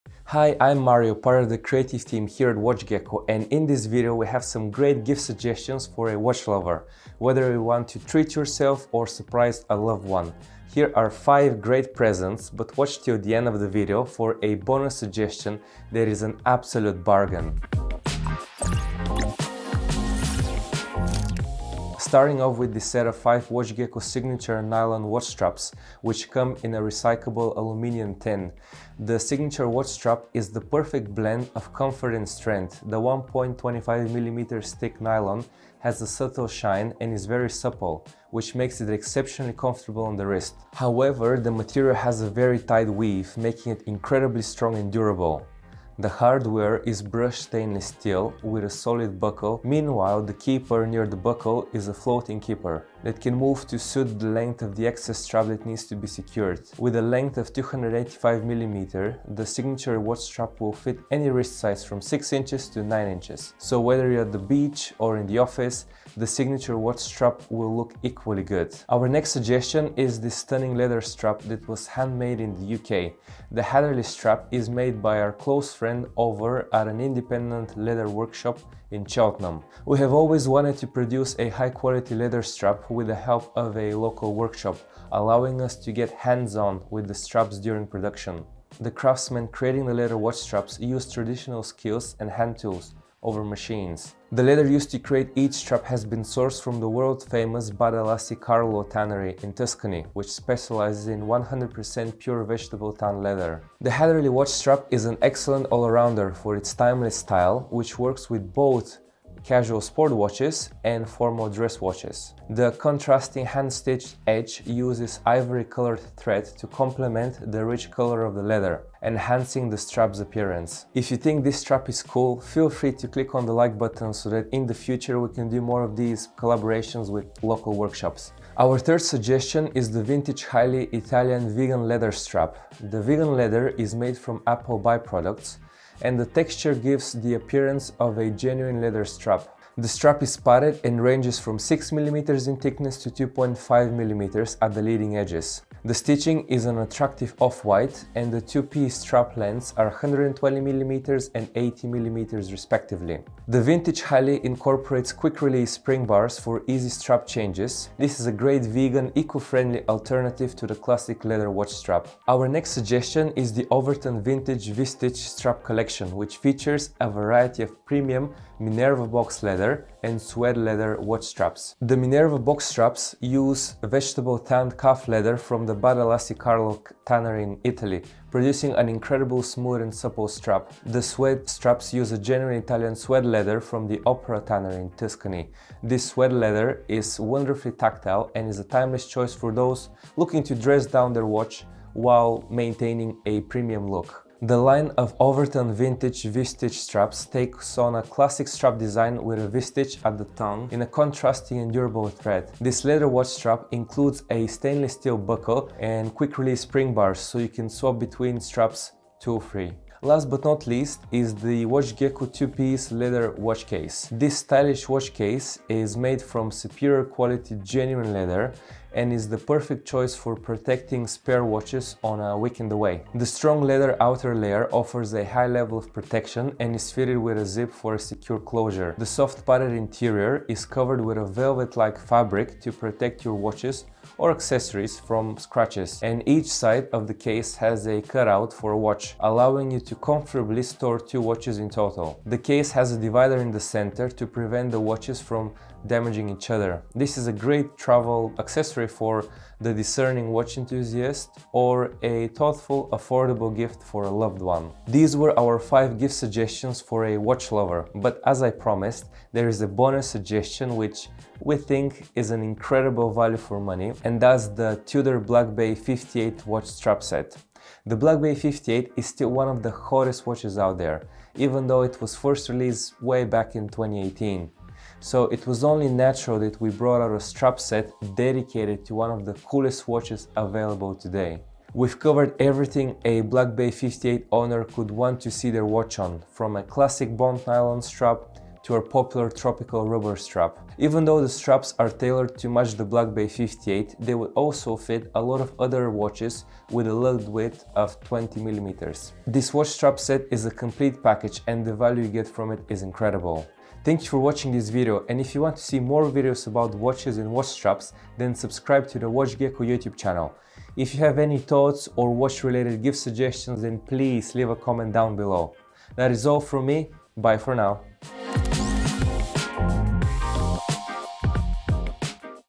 Five AWESOME gift ideas for watch lovers!
Here at WatchGecko, we had a brainstorming session on finding some great gift suggestions for watch enthusiasts. We are passionate about watches and know how a high-quality watch strap can bring new life to your favourite watch. So if you're wondering how to treat yourself or surprise a loved one then have a look at some of our best straps suggestions.

Model Wrist Size: 170mm

0:00 Intro
0:22 Signature
1:09 Hatherley
2:09 Vegan Highley
2:43 Overton
3:31 Watch case
4:18 Bonus gift idea